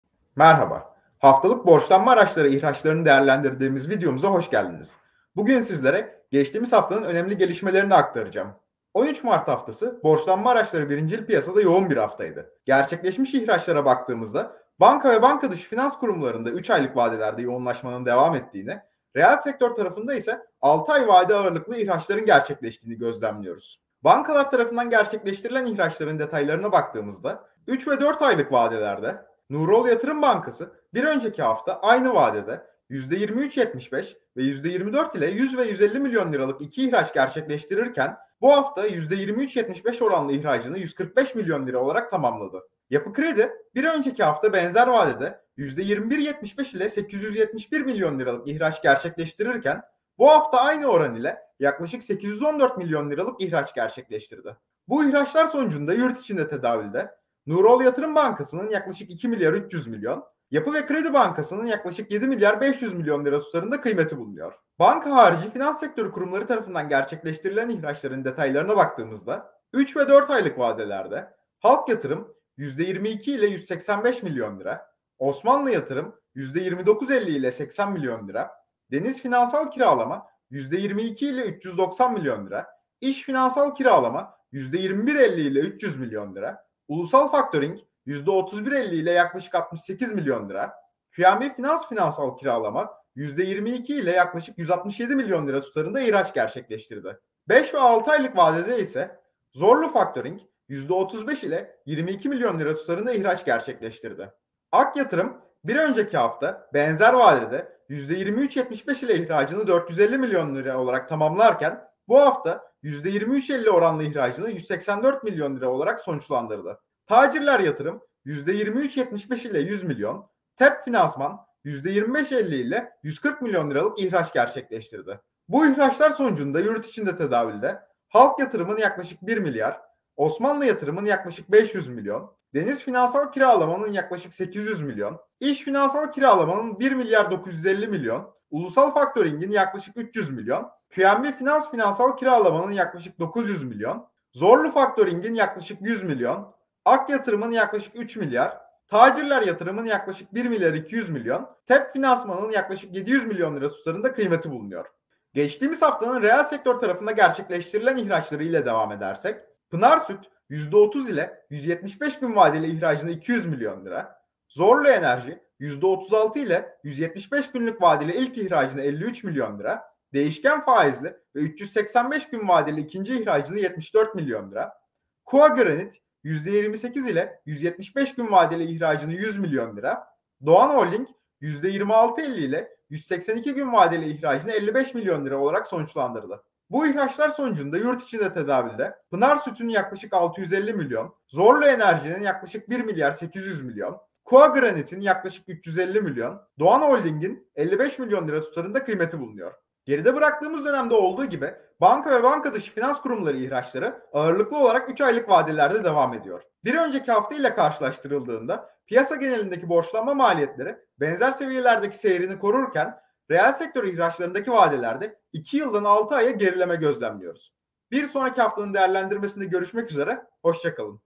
HAFTALIK BORÇLANMA ARACI İHRAÇLARI - 20.03.2023 - Metin Sehim
📌 İş Yatırım Kurumsal Finansman Müdürlüğünden Metin Sehim, geride bıraktığımız haftanın borçlanma aracı ihraçları hakkında bilgiler paylaşıyor.